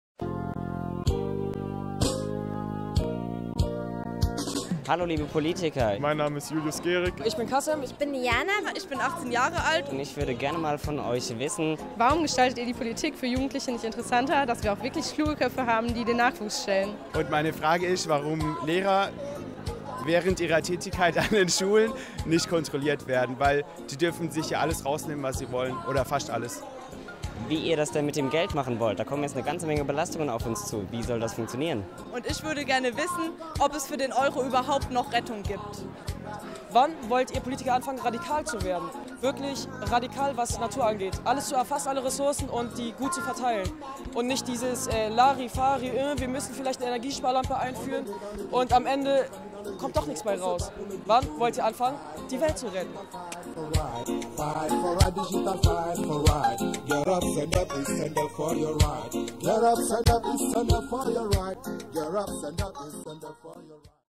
Zugehört! Eure Fragen an die Politik
DU HAST DIE MACHT hat auf den Jugendmedientagen 2011 in Stuttgart die jungen Teilnehmer gebeten, ihre Fragen an die Politik loszuwerden -- mit dem Versprechen, die Antworten darauf einzuholen. Und zwar am 29. Februar im Rahmen des Events ZUGEHÖRT in Berlin. Jugend trifft Politik. Und dann geht's zur Sache.